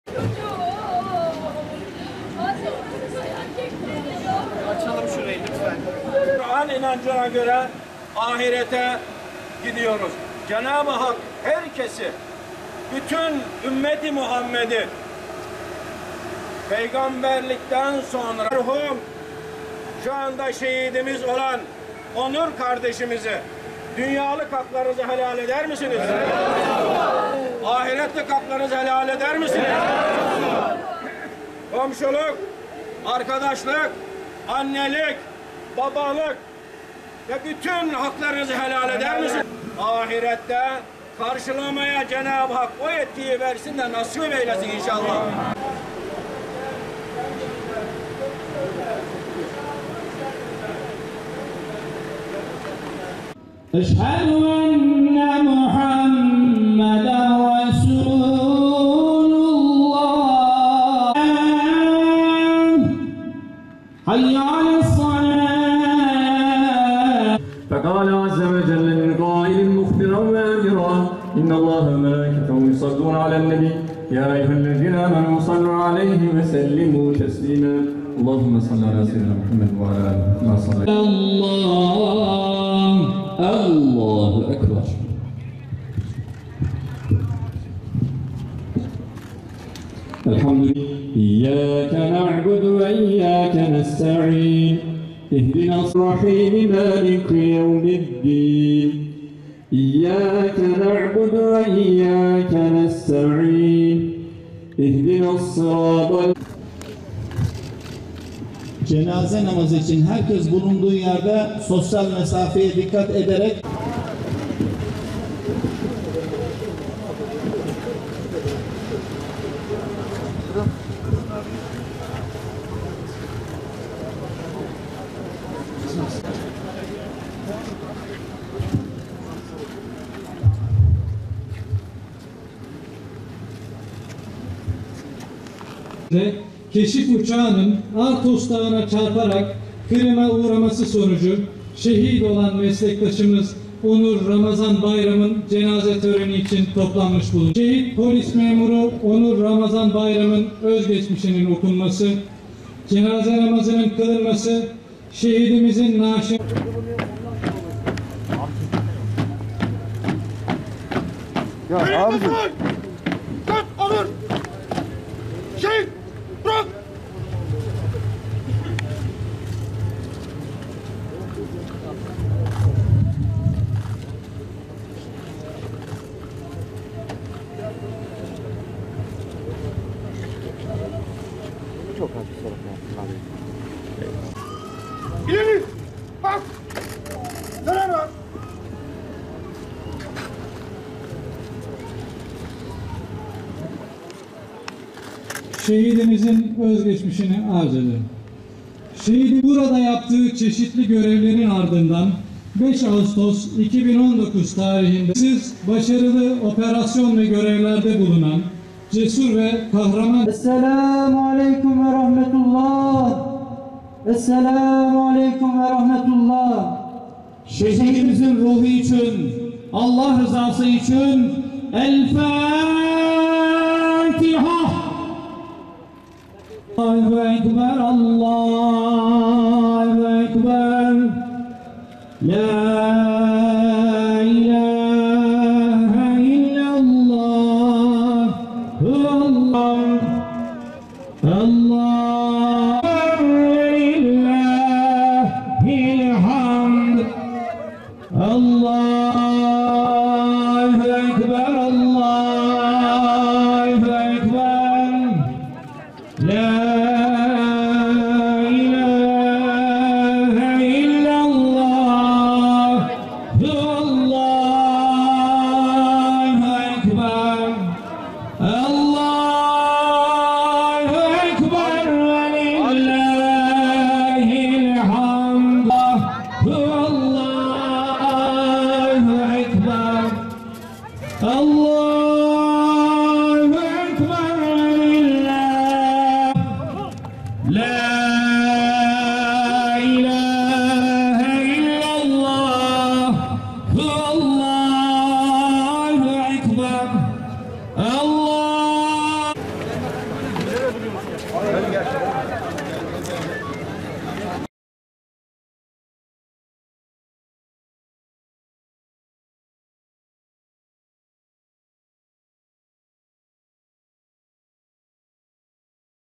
O ilimiz, bir hafta içinde ikinci şehidini ebediyete uğurladı
Van’dan kalkan keşif uçağının görevden dönerken kırıma uğraması sonucunda şehit düşen özel harekat polisi Onur Ramazan Bayram, memleketi Manisa’da düzenlenen törenle toprağa verildi.